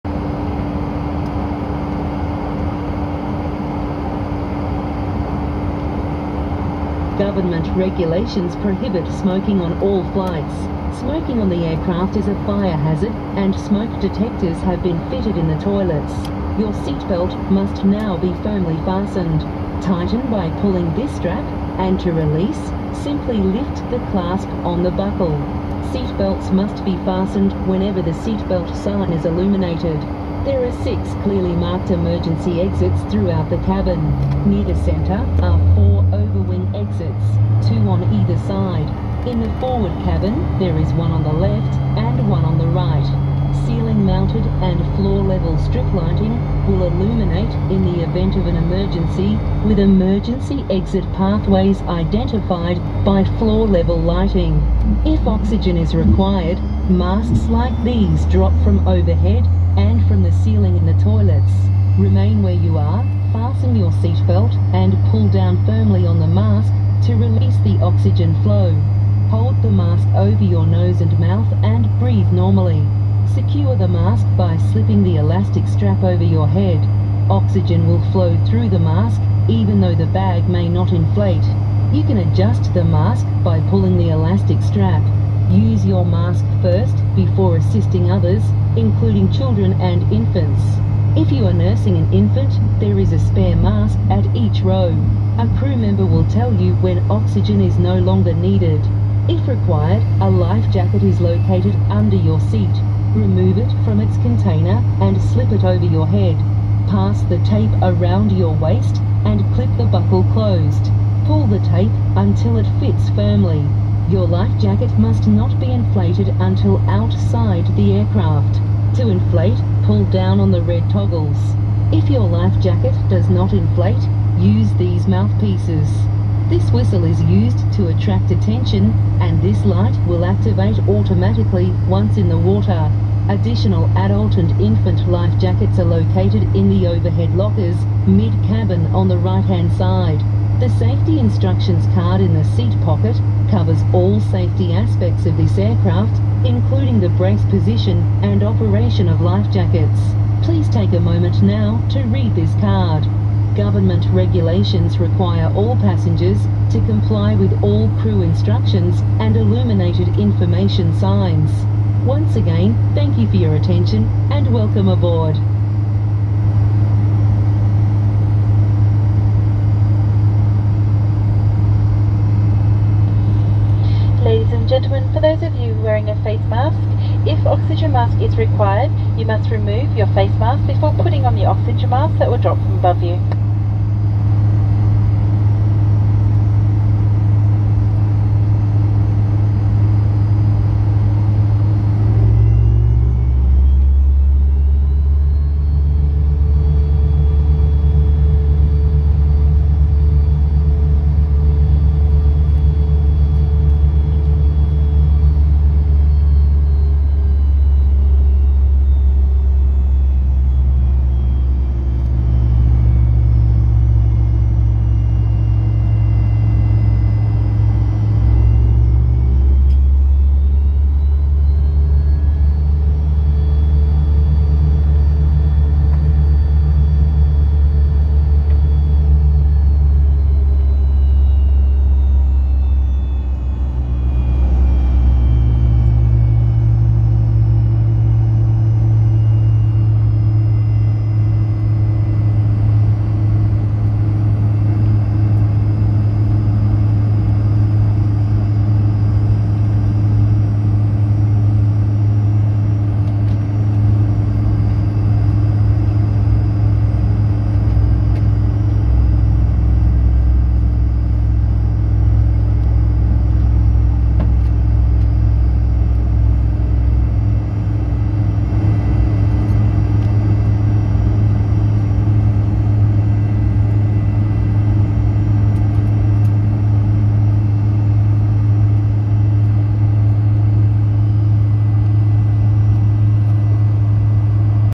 Alliance Airlines Fokker 100 VH-XWT QQ3240 Pushback and Engine Start at Adelaide
Humble beginnings for this flight to Olympic Dam from Adelaide Airport. Shot from seat 18G